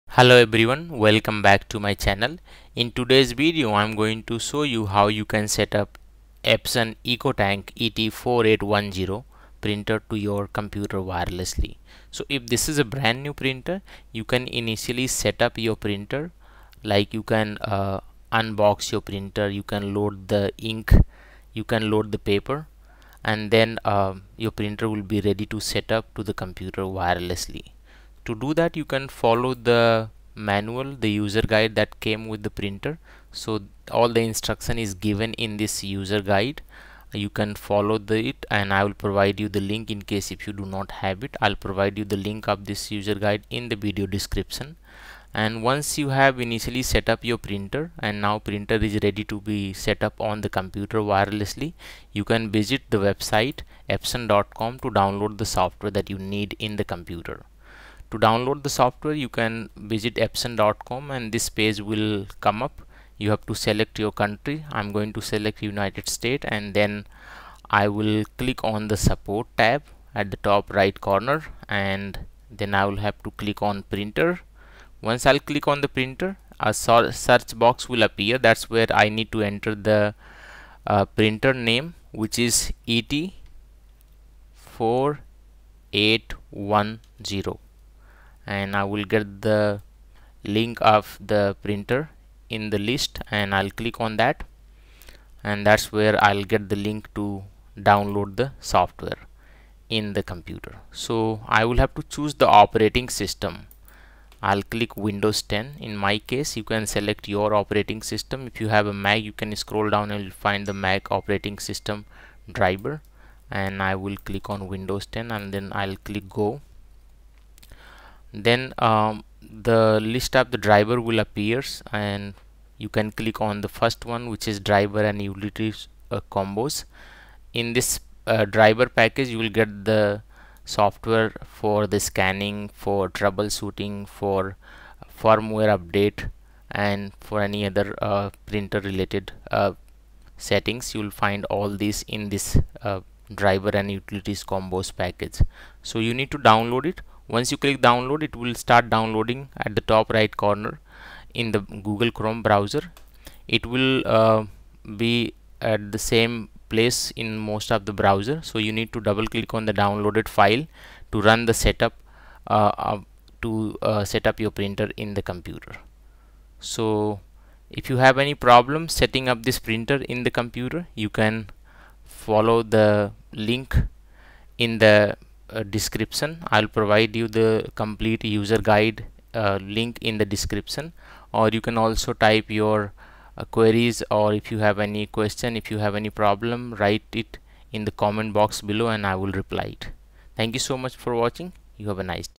How to Setup Epson ET 4810 in Easy Steps
In this video, we’ll walk you through the complete process of setting up your Epson ET 4810 EcoTank All-in-One Printer from start to finish.

If you're facing any issues setting up your Epson ET 4810 printer or need further assistance, feel free to reach out to us for personalized help. to get in touch with our support team. We're here to help you with any printer problems you may have!